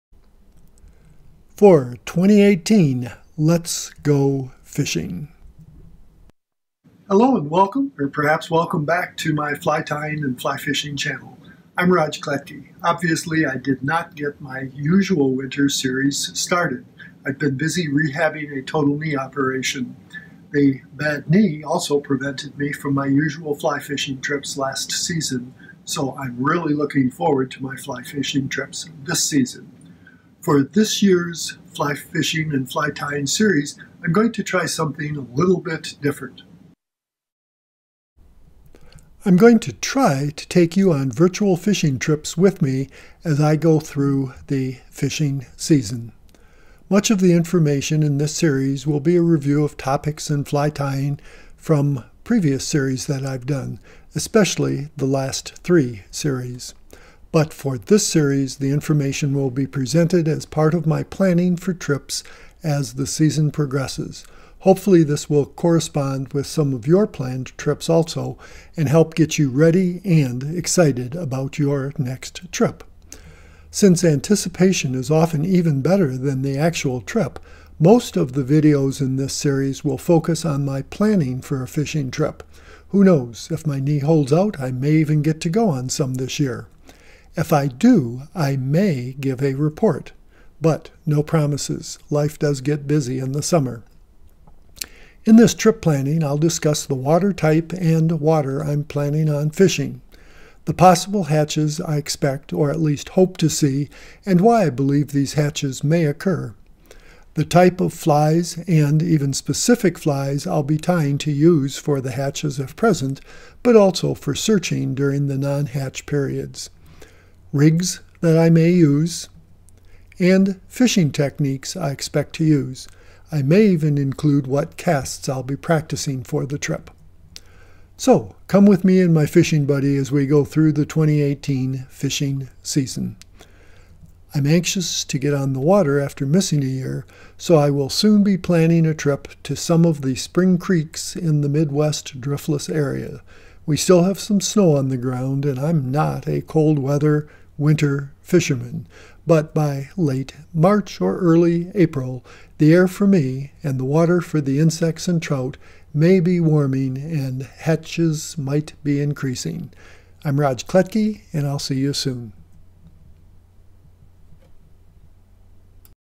Introduction to the 2018 fly fishing series
This will be my 2018 series in which I hope to "take you fishing with me" as the season progresses.